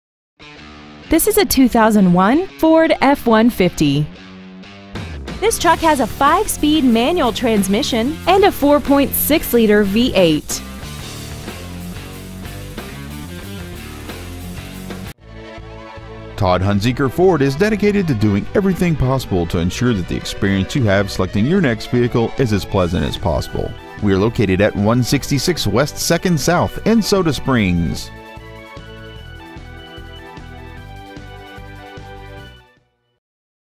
2001 FORD F-150 ID
We have been honored to serve the  ID area , we promise that your experience at our dealership will exceed your expectations !

 Year : 2001
 Make : FORD
 Model : F-150
 Engine : 4.6L V8 16V MPFI SOHC
 Trans . : 5-SPEED MANUAL
 Exterior : BRIGHT RED CLEARCOAT
 Miles : 87,995
 Interior : DARK GRAPHITE
 Stock : F11U622

 166 West 2nd South
  , ID 83276